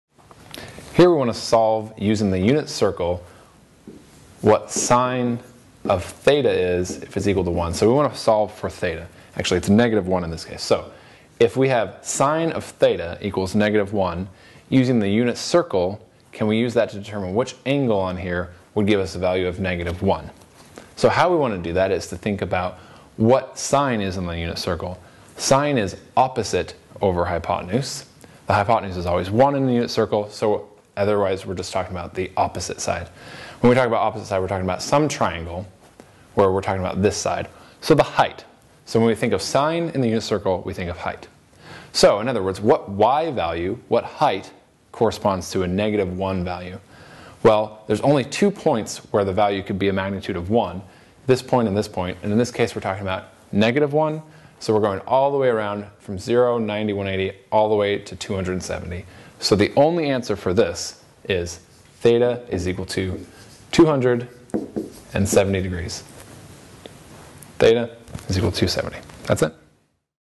Circular Functions
In this video, we explore how to solve for sine using the unit circle when the value is negative one. By thinking about the height or Y value corresponding to a negative one, we can determine the angle on the unit circle that gives us this value. In this case, the answer is theta equals 270 degrees. Join us to learn more about using the unit circle in trigonometry!